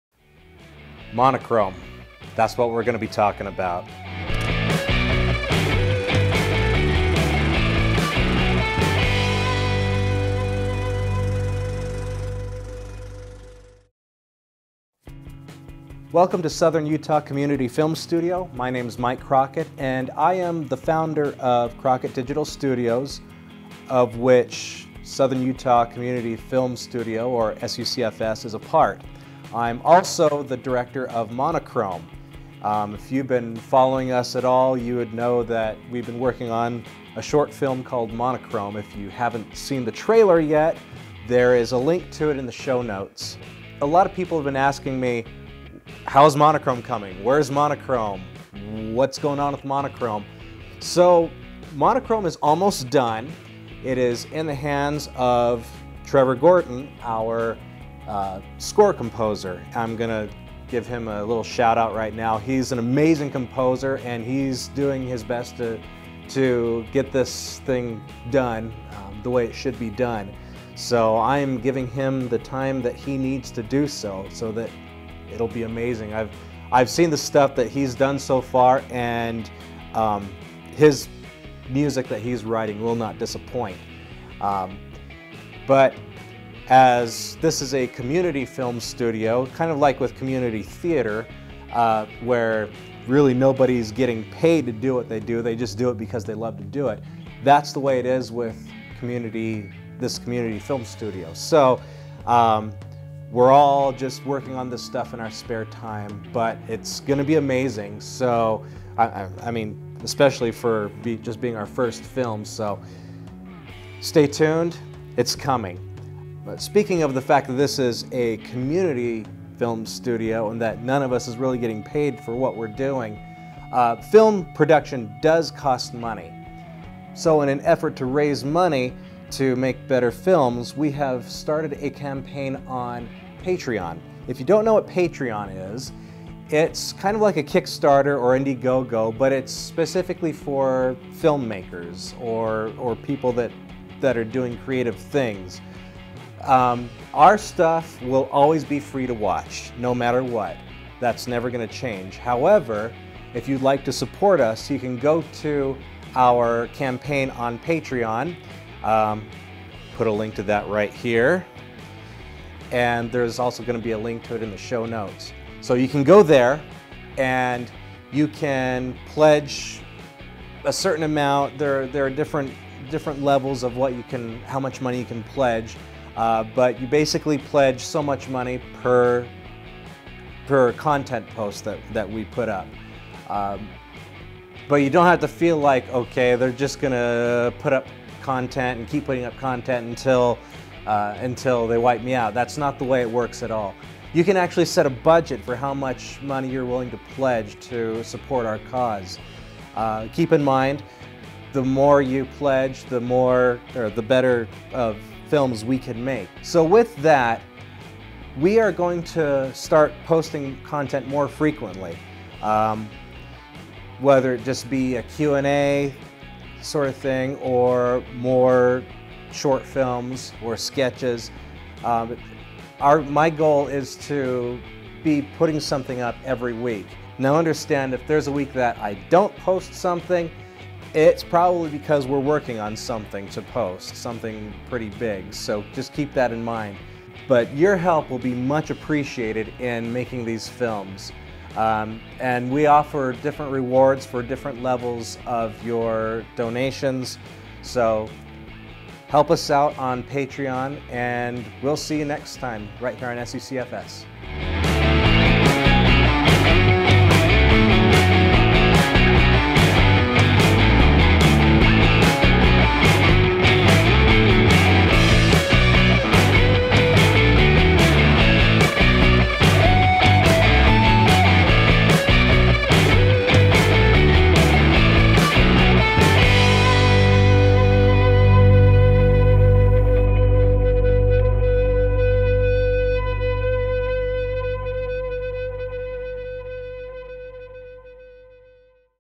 Where's Monochrome?
Mike Crockett talks about Monochrome, the first film from SUCFS, which will be coming out soon.

Tweet at Mike:  @mike_mcrockett